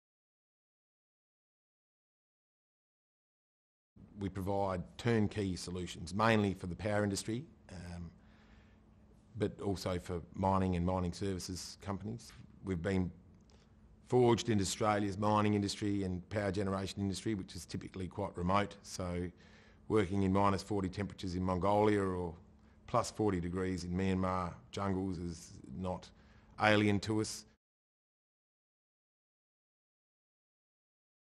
RJE Global Export Award Finalist 2016
RJE Global national finalist in the 54th Australian Export Awards (Manufacturing category).